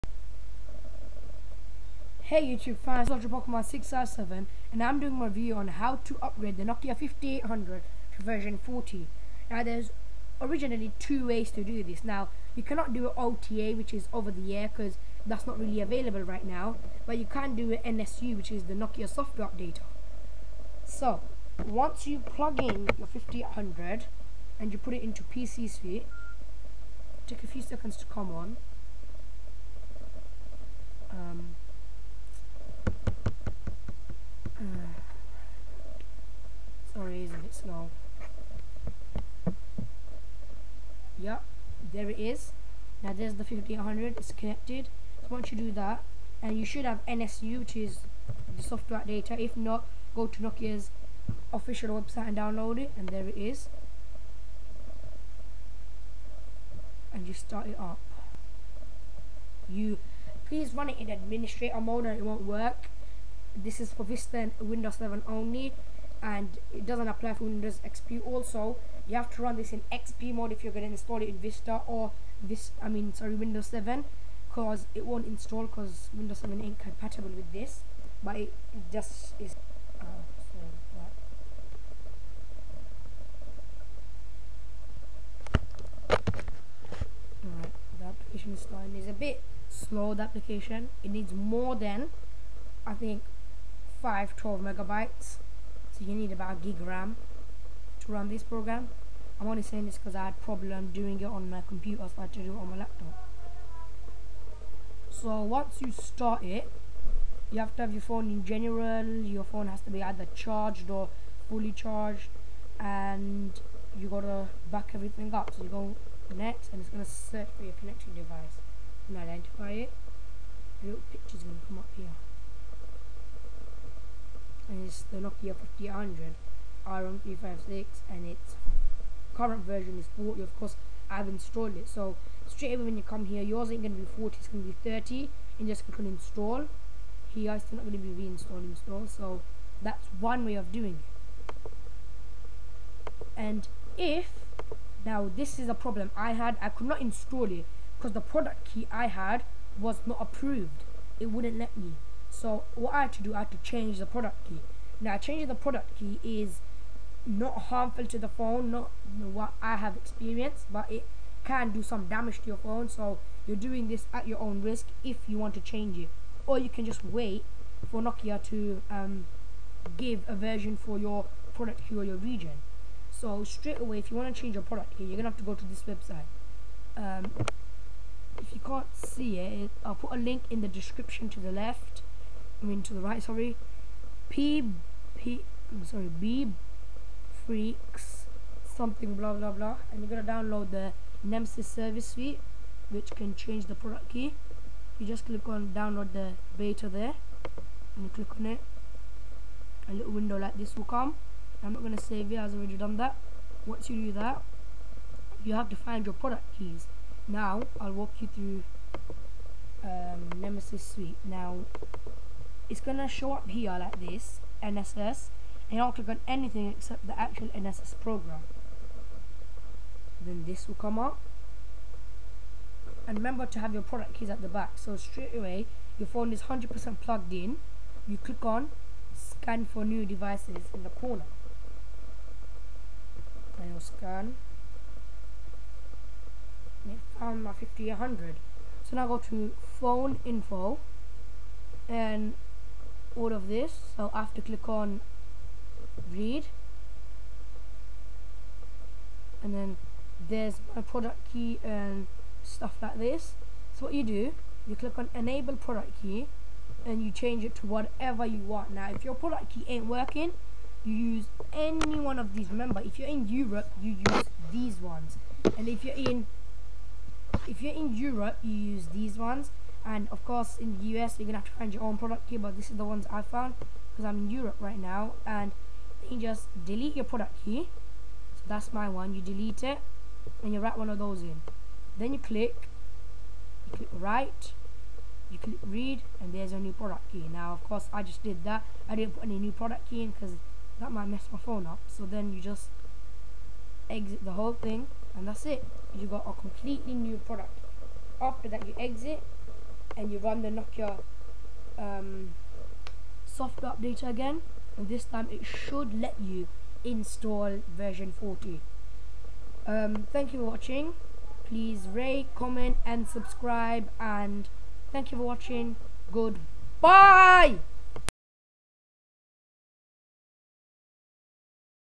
How to install v40 on nokia 5800
How to install v40 on nokia 5800 this is for people who are finding it hard to install or thier product key is not supported.
 this link is for nss